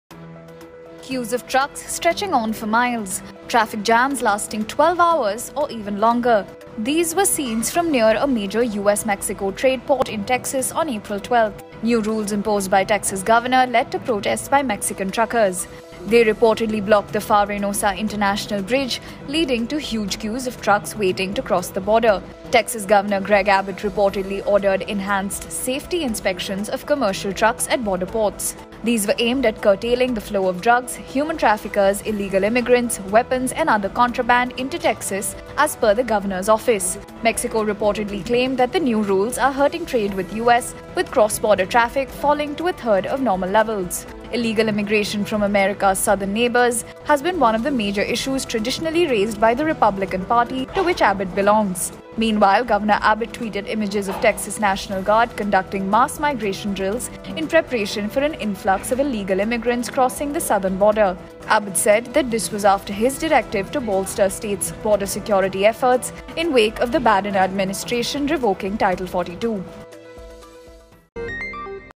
US: 12-hour long truck jams in Texas. Watch why new border rules led to protest at Mexico trade port
Queues of trucks, stretching on for miles. Truck jams lasting 12 hours, or even longer. These were scenes from near a major US-Mexico trade point in Texas on April 12.

Watch the video to know why new border rules led to protest at Mexico trade port.